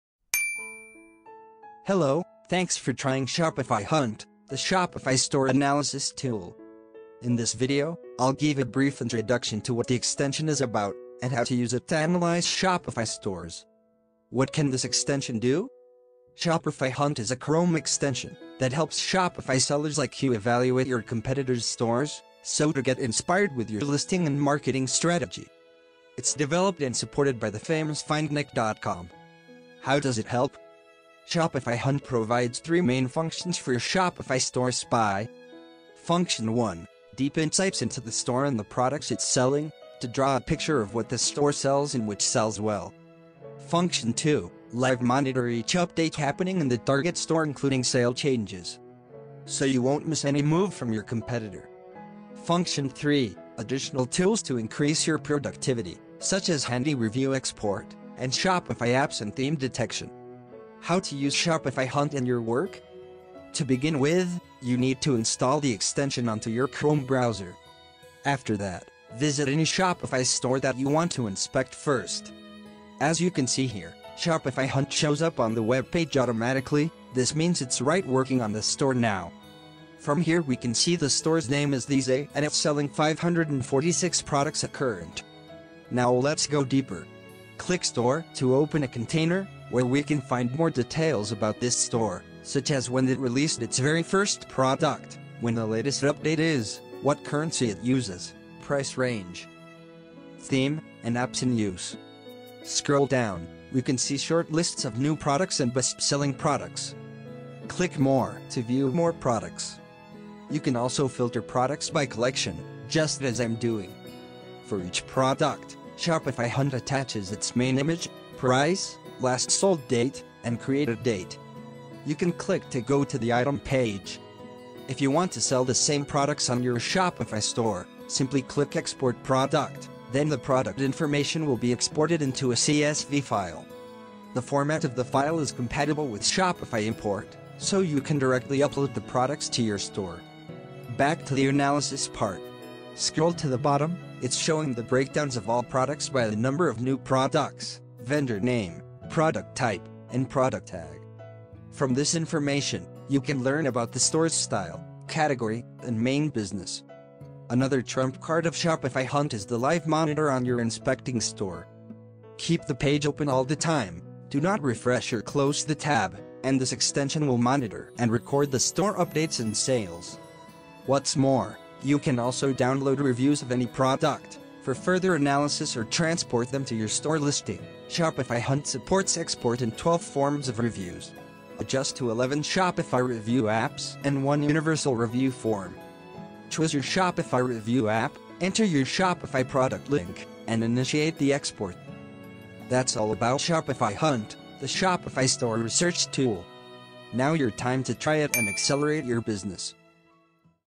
How To Monitor Shopify Stores with ShopifyHunt?
This is a guide video of ShopifyHunt, a free Chrome extension that collects and analyzes all the information about target Shopify stores and products.